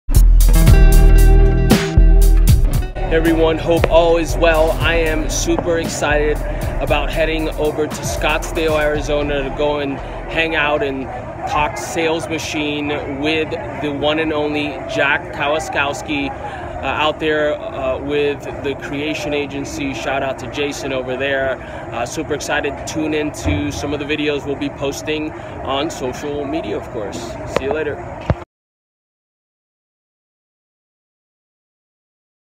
🎥: Road to SalesMachine17 w/ Jack Kosakowski & Jorge Soto
Jorge Soto of Freedeo & Jack Kosakowski of Creation Agency talk shop leading to SalesMachine17 in NYC!